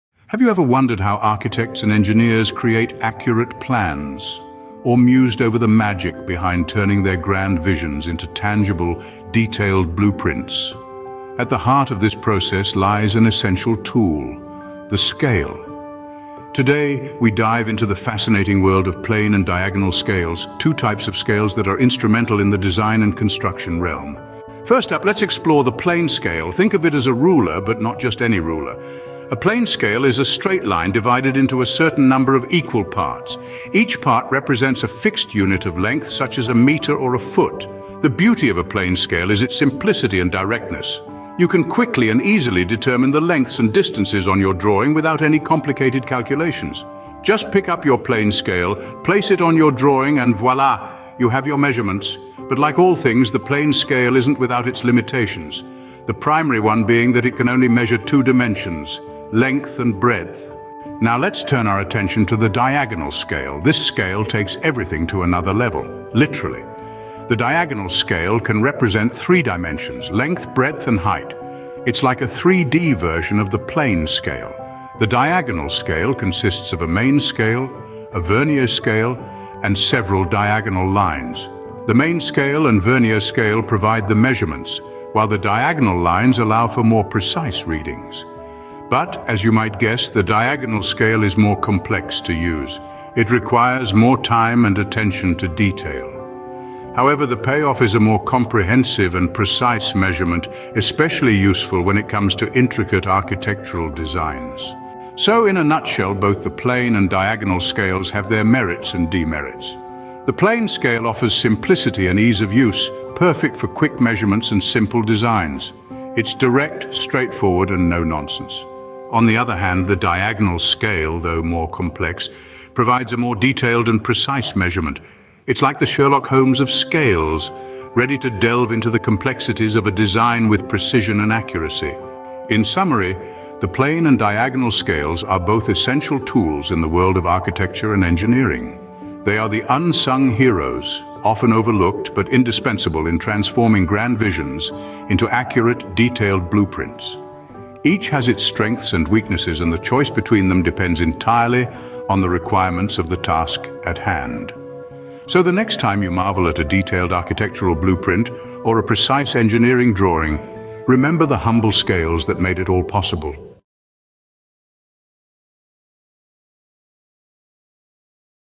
INTRODUCTION TO SCALE DRAWING. PRE-TECHNICAL STUDIES. GRADE 8.
*STRAND* COMMUNICATION*
*SUB-STRAND* PLANE SCALE DRAWING.
INTRODUCTION TO SCALE DRAWING.
PRE-TECHNICAL STUDIES.
GRADE 8. CBC KENYA.